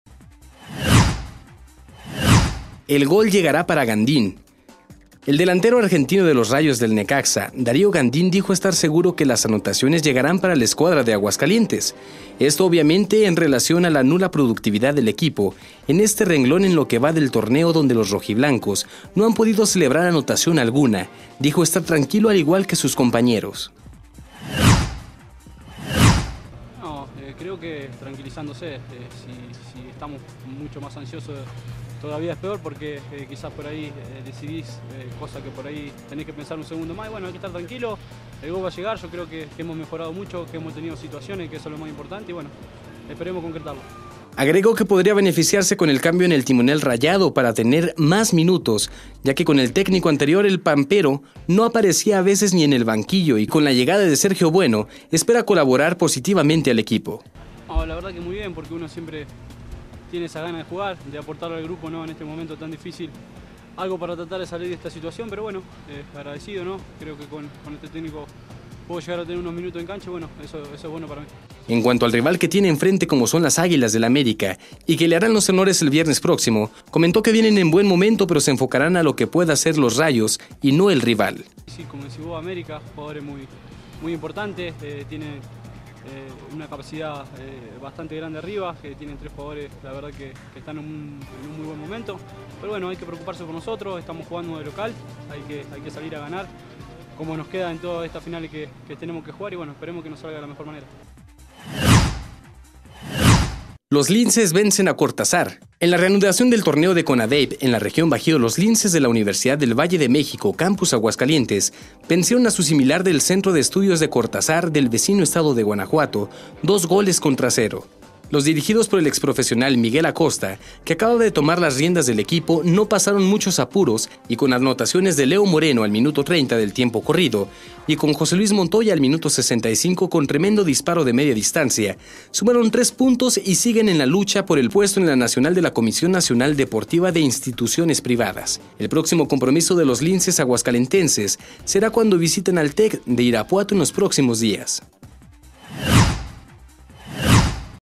Deportes 080211
Dar&iacute;o Gand&iacute; asegura que el gol del Necaxa llegar&aacute;.